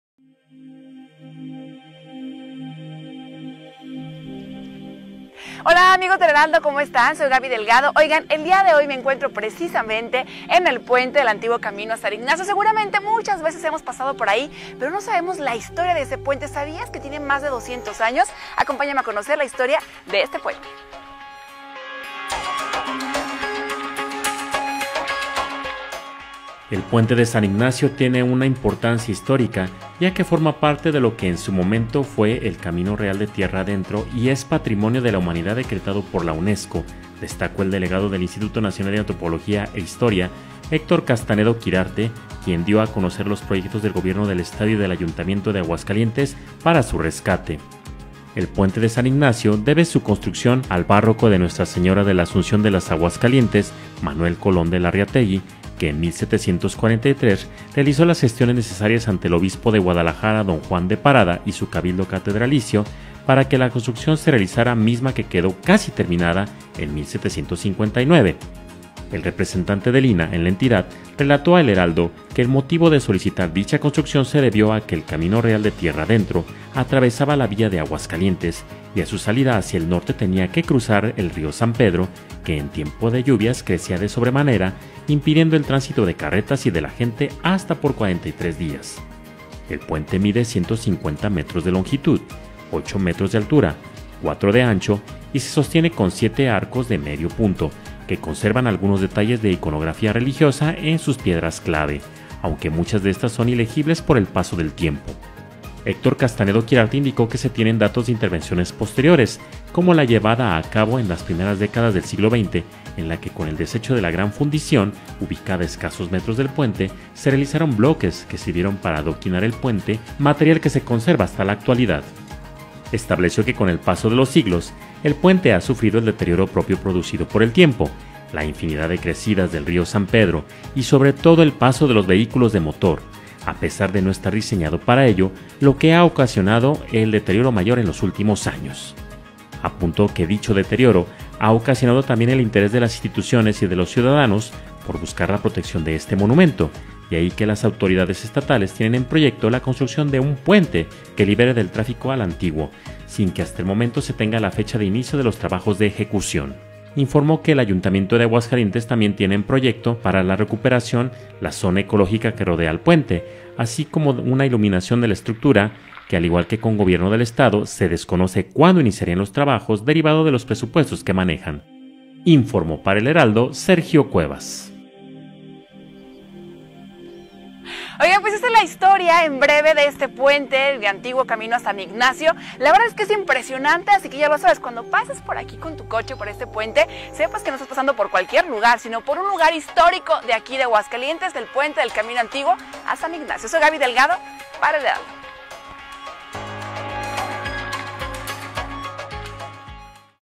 Tierra Adentro: Puente de San Ignacio
El Puente de San Ignacio tiene una importancia histórica, ya que forma parte de lo que en su momento fue el Camino Real de Tierra Adentro y es Patrimonio de la Humanidad, decretado por la UNESCO...